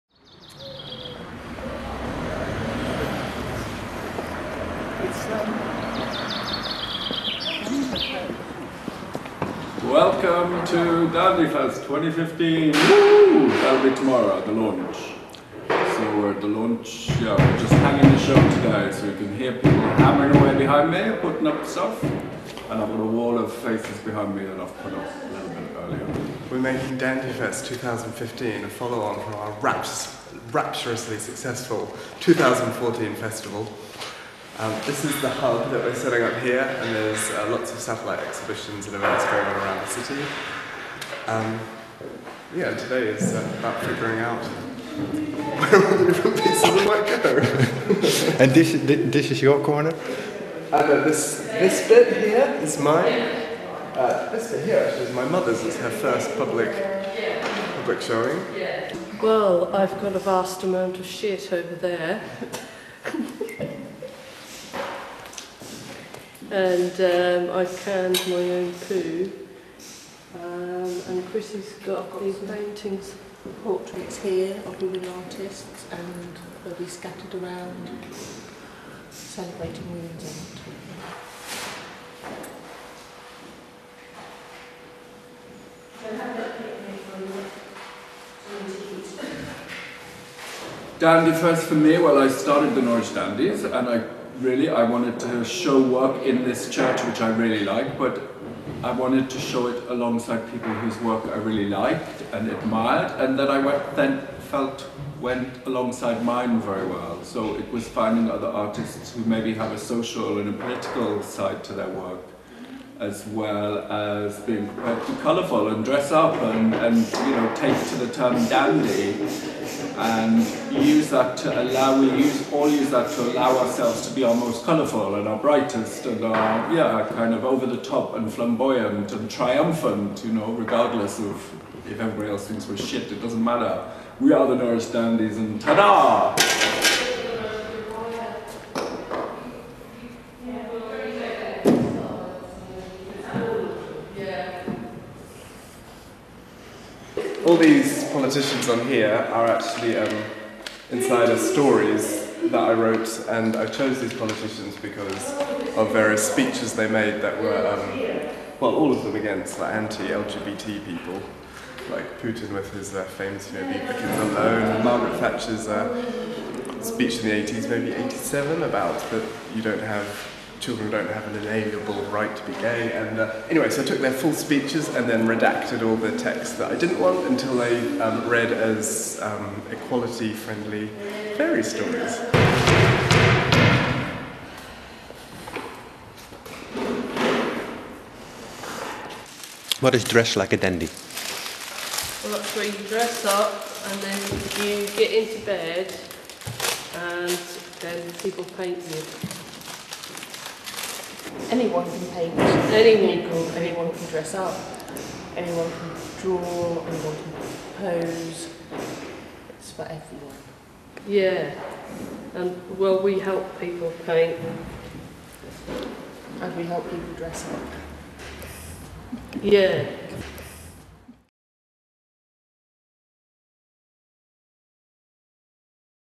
Installing Dandifest 2015
Vince Laws, Dugald Ferguson, Chrissy Sabberton and Eloise O'Hare installing a Dandifest exhibition at St Margaret's Church of Art in Norwich.